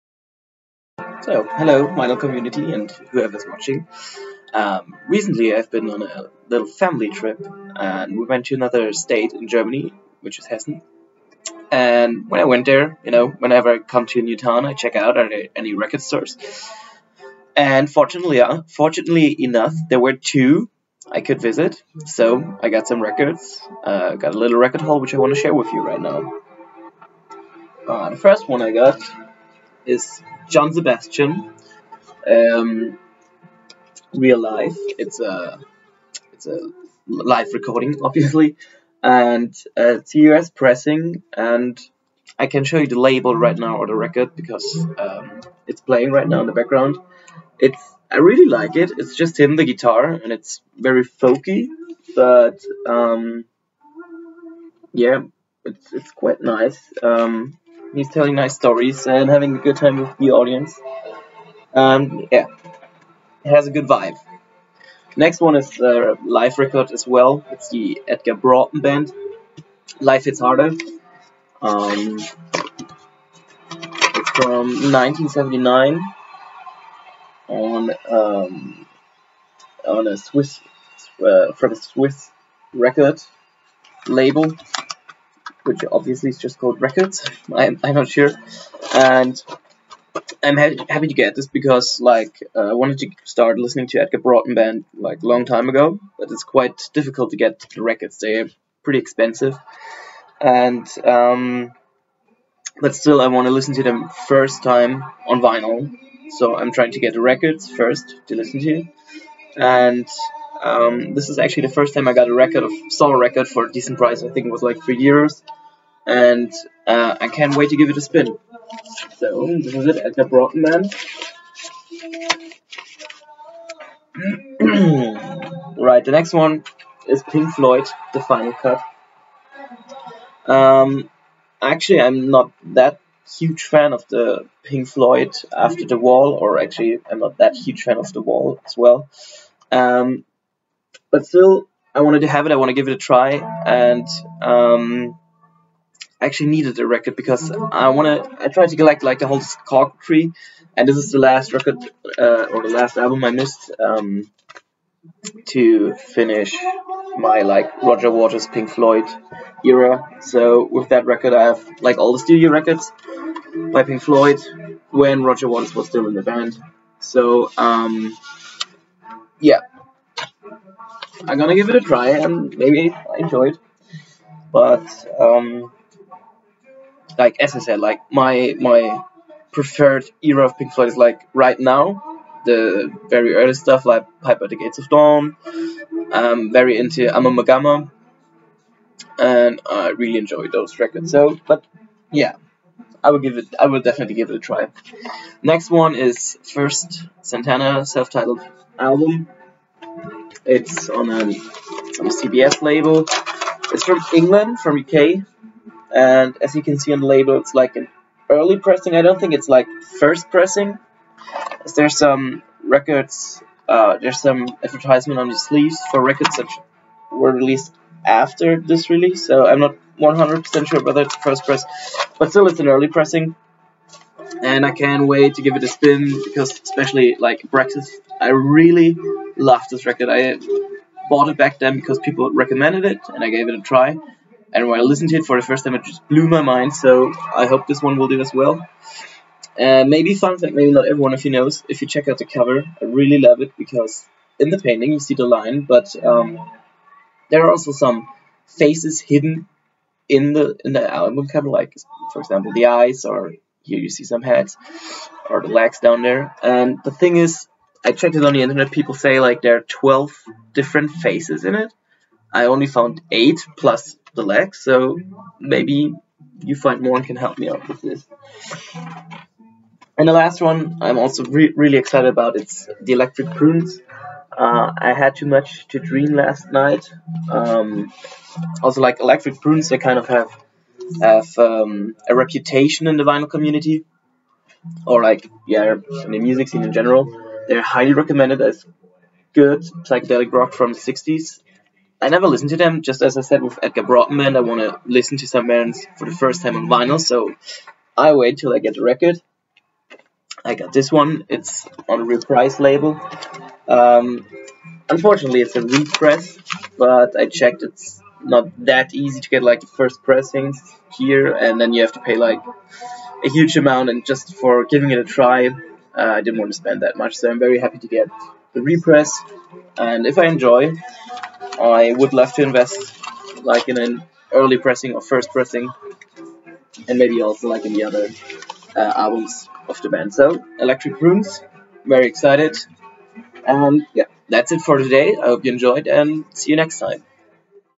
#2 Record Haul: Hessen Haul with Pink Floyd, Electric Prunes etc.
I went to Hessen and checked some record stores. This is what I found.